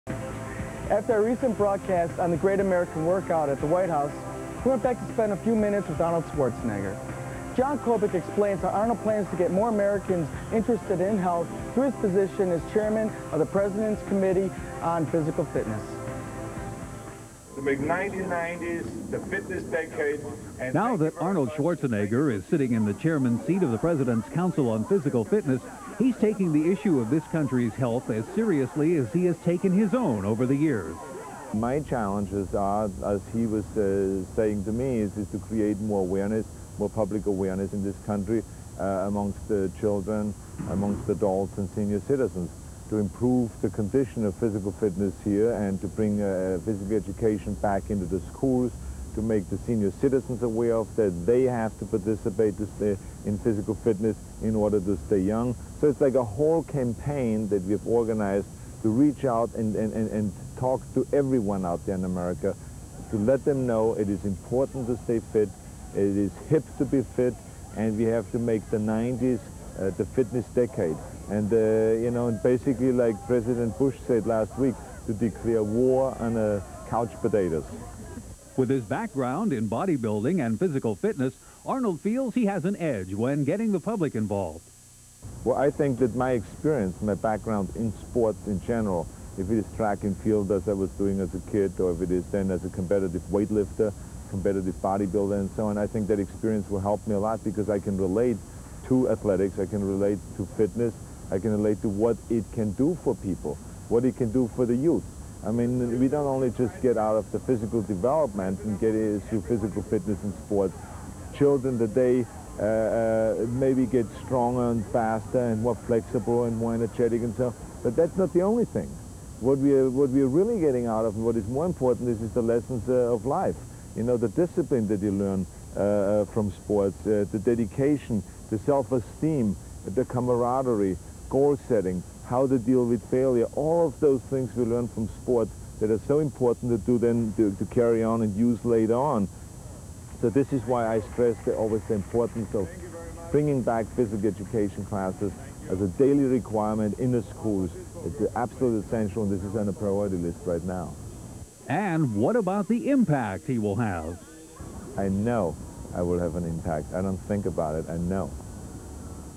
Arnold Schwarzenegger Meets President George H W Bush 1990
Clip from 1990 introduced by the late Pro Bodybuilder Vince Comerford - 43rd United States President George Herbert Walker Bush (Reagan's VP) appointed Arnold Schwarzenegger Chairman of his Council on Physical Fitness.  Not sure if that Council is still around, but Arnold's interest in politics may not be over yet...